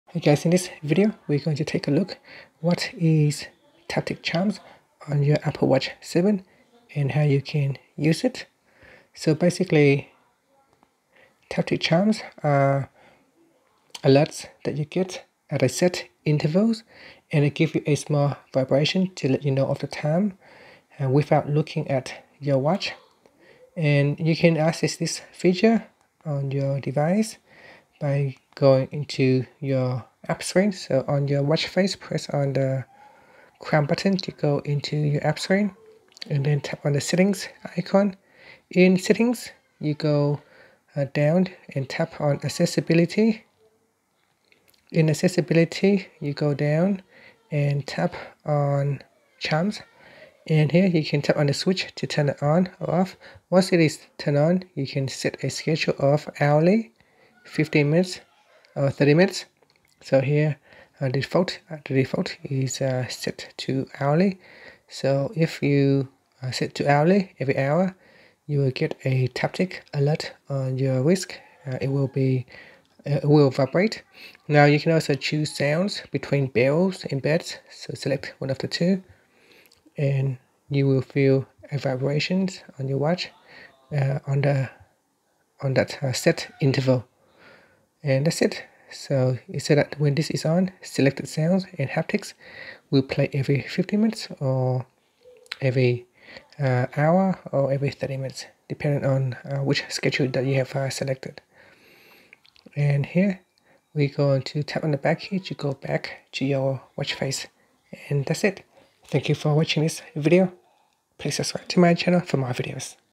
What is Taptic Chimes on the Apple Watch 7
See what is Taptic Chimes on the Apple Watch 7 and how you can use it.